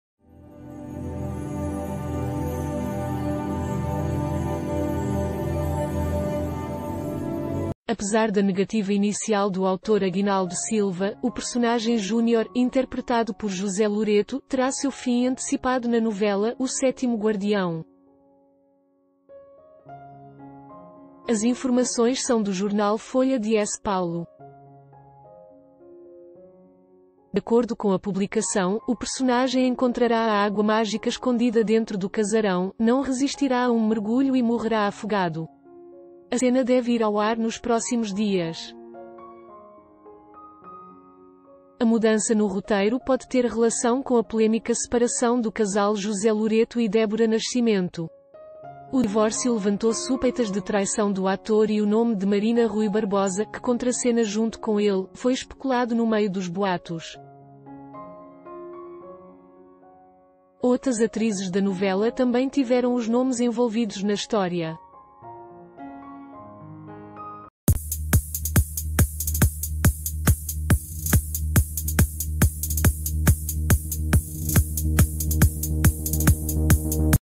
Personagem de José Loreto morrerá afogado em 'O Sétimo Guardião', diz jornal - ISTOÉ Independente
Apesar da negativa inicial do autor Aguinaldo Silva, o personagem Júnior, interpretado por José Lore...